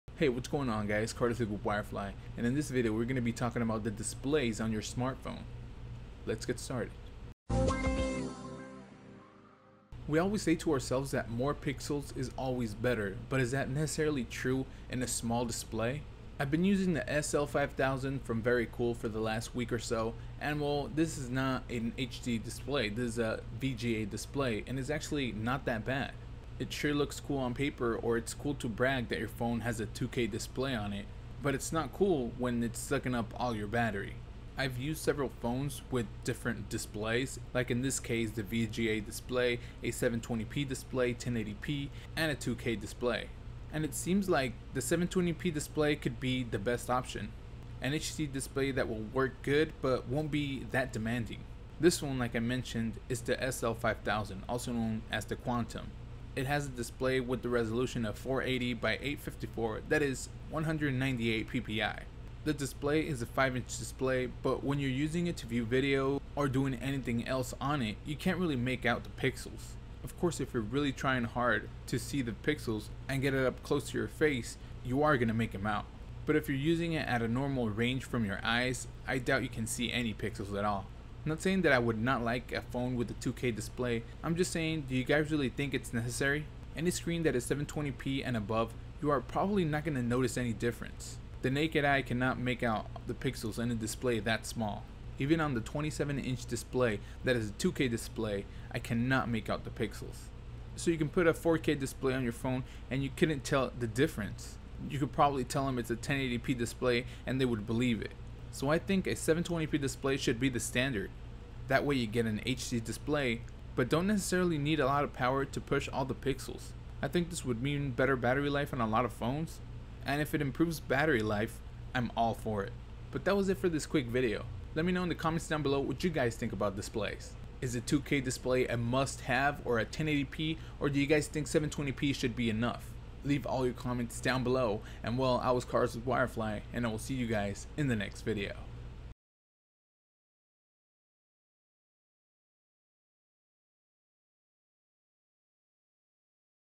Cell Phone Displays: Why Resolutions Matter
Wirefly talks about the different resolutions there are and how it affects your phone. We could stick to a certain resolution since after that we cannot see pixels anymore.